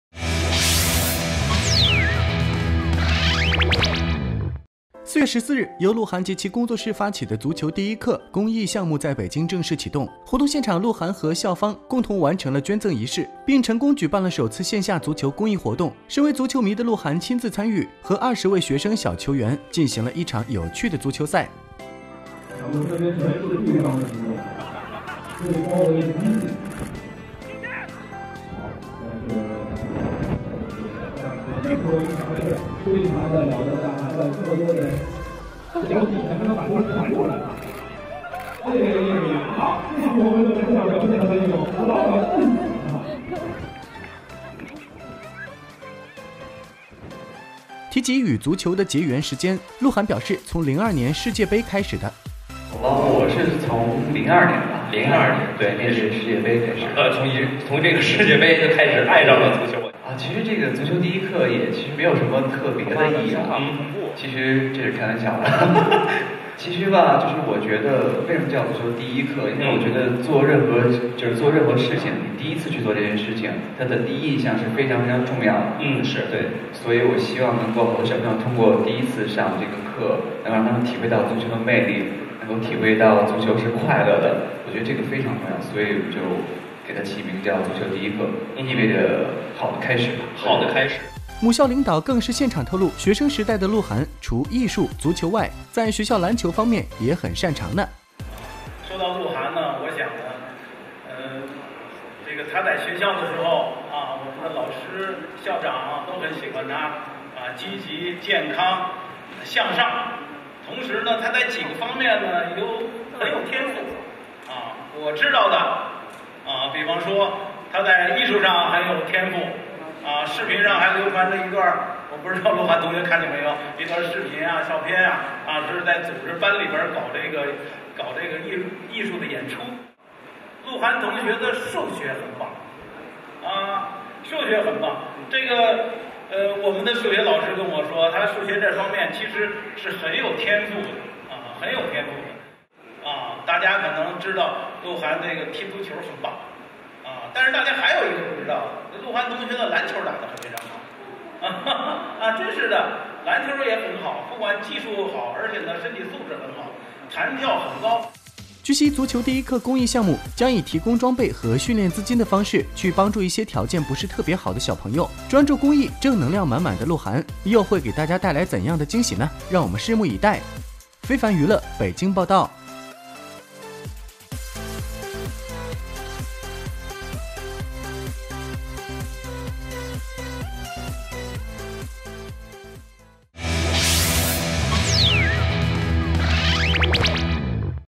鹿晗投身足球慈善公益活動 期望盡自我所能助孩子完成夢想
4月14日，由鹿晗及其工作室發起的“足球第一課”公益專案在北京正式啟動，活動現場鹿晗和校方共同完成了捐贈儀式，并成功舉辦了首次線下足球公益活動。身為足球迷的鹿晗親自參與活動，和20位學生小球員進行了一場有趣的足球賽。